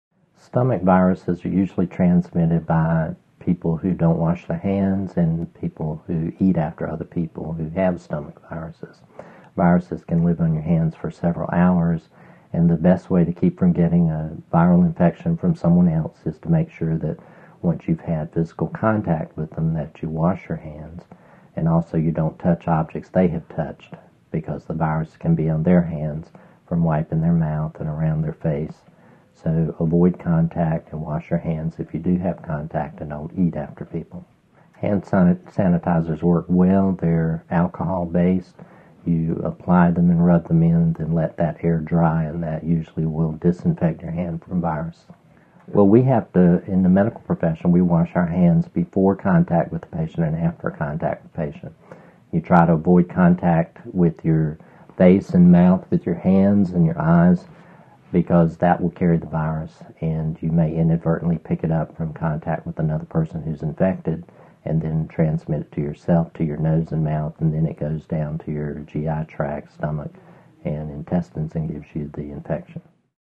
How Is the Stomach Virus Transmitted?
How Is the Stomach Virus Transmitted?. Part of the series: Medical Treatments & Answers. The stomach virus is typically transmitted by people who don't wash their hands and people who eat after others who have stomach viruses themselves. Discover how viruses can live on the hands for several hours with help from a physician's assistant in this free video on stomach viruses.